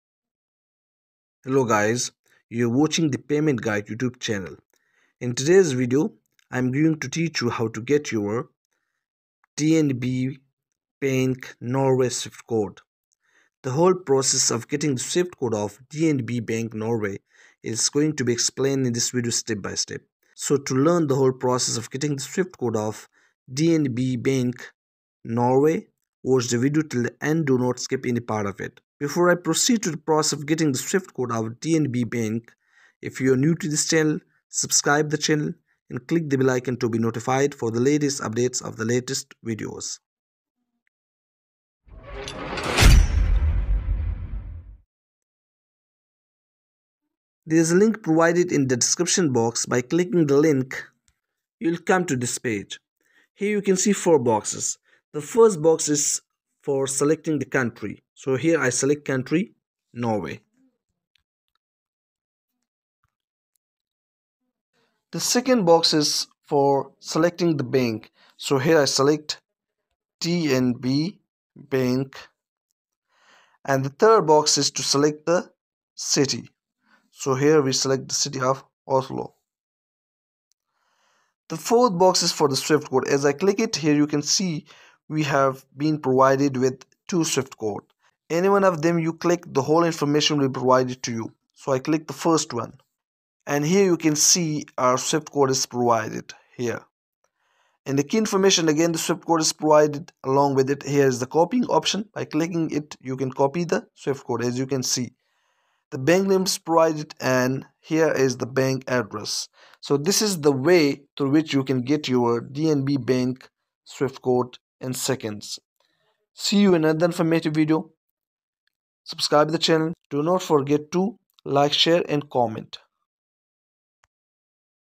How To Get DNB Bank SWIFT CODE (BIC) | Payment Guide
In this video, you will learn how to get your DNB Bank of Norway Swift code or BIC to use wherever you need it.
So, to learn how to get  the DNB Bank of Norway Swift code watch this video till the end without skipping any part of it.